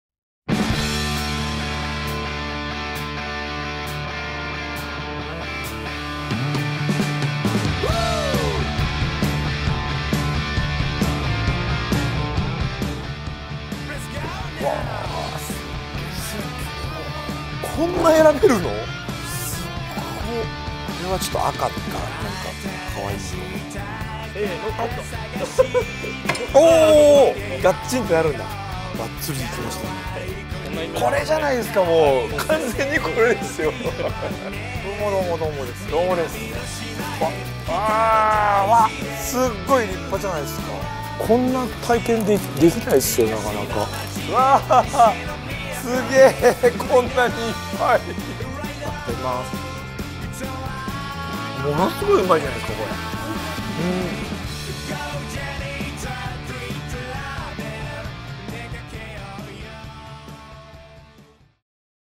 廣瀬“HEESEY”洋一 オフィシャルファンクラブ『WE☆SAY』限定企画が遂に始動！
HEESEYのオフィシャルファンクラブ「WE☆SAY」の皆様からHEESEY にやってほしいこと募集しました「DISCOVER JAPAN」が遂に始動いたします！
第一弾は岡山県倉敷市児島にある「Betty Smith」さんへお邪魔しオーダージーンズの作製とポーチへのリベット打ち体験を行いました。
第二弾では会員の方の農園「スミヤ果樹園」さんへお邪魔し果実の収穫のお手伝いをさせていただきました。
第一弾は2022年1月公開予定。
是非お楽しみに！

■2022年1月配信予定『オーダージーンズ製作』
Betty Smithさん

■2022年3月配信予定『果実の収穫手伝い』
スミヤ果樹園さん

配信予定のコンテンツはWE☆SAY会員限定になります。

廣瀬“HEESEY”洋一　OFFICIAL FAN CLUB「WE☆SAY」会員募集中!!
ご入会はこちらから。